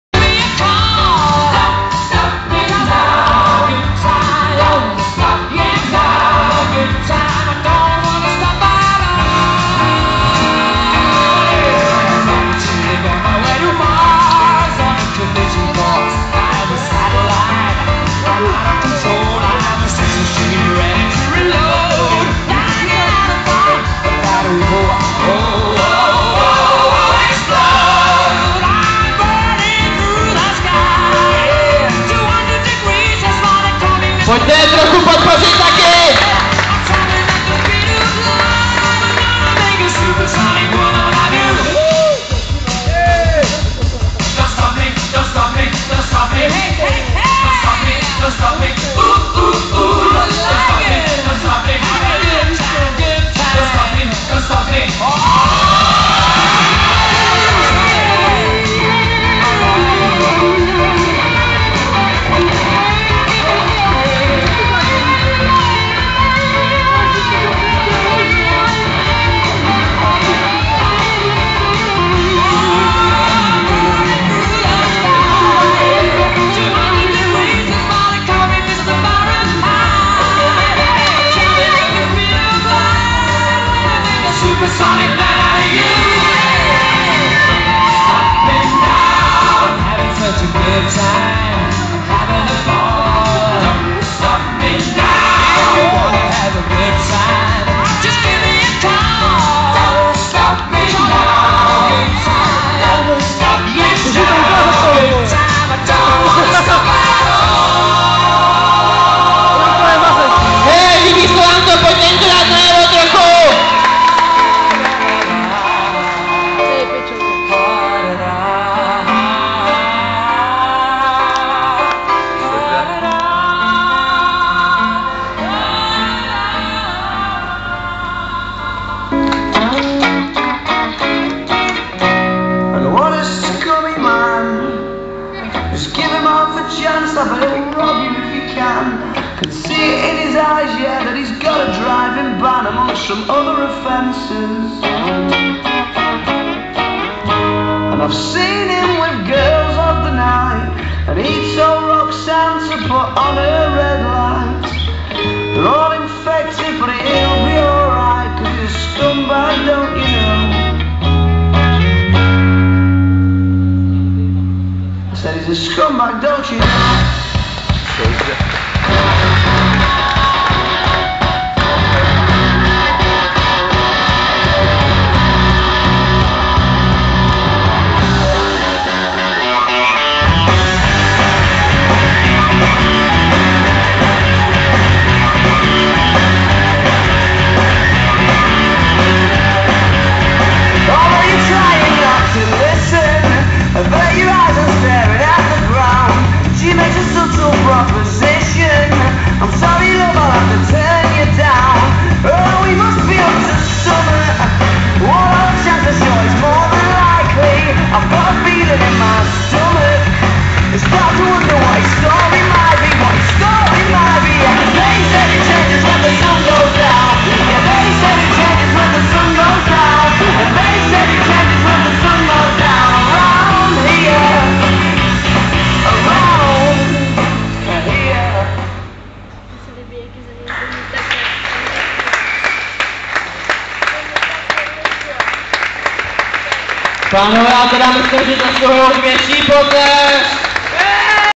FIFA.cz 5th B-day Party footbag exhibition
Vasek Klouda na 5 narozeninach webu FIFA.cz. Moc loupacky.